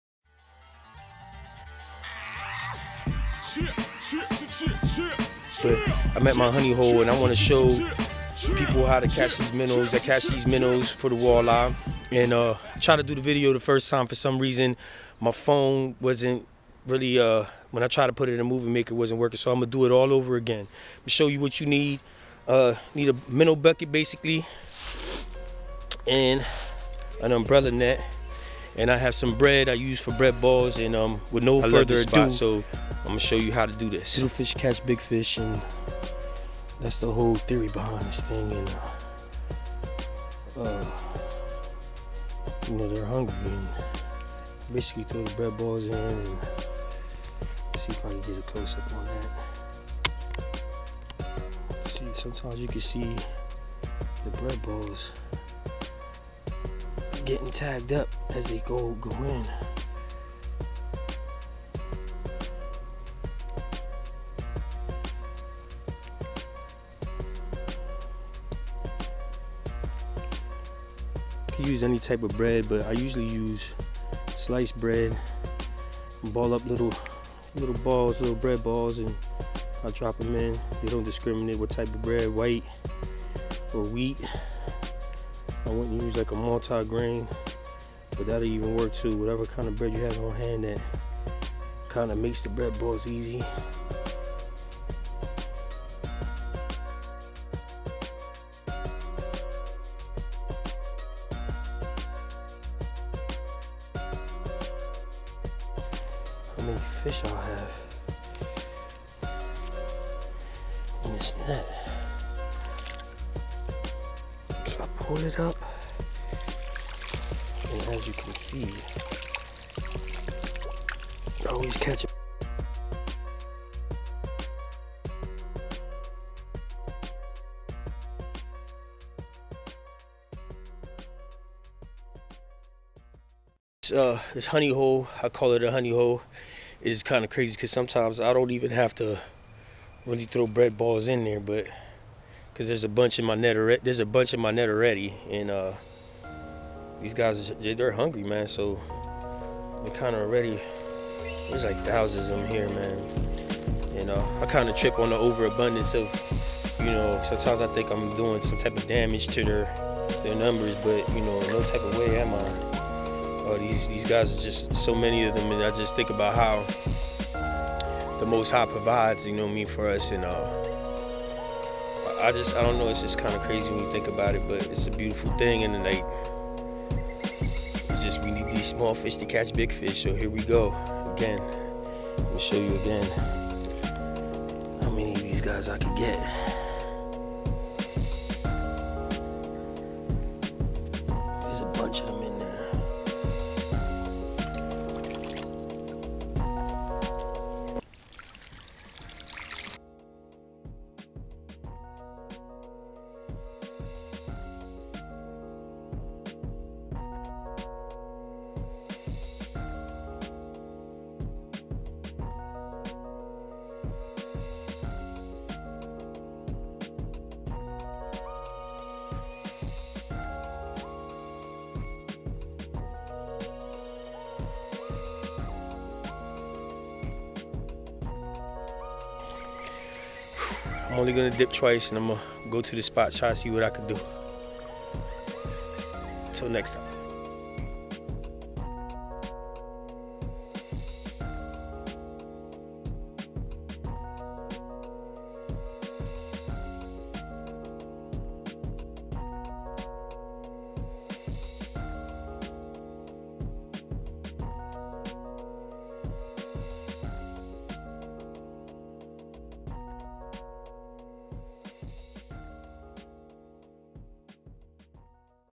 How to Catch Shiners!
Basically showing the world how i catch shiners. you can spend a little money and time to catch your own and will save lot's of money in the long run you can thank me later. or now by subscribing!!!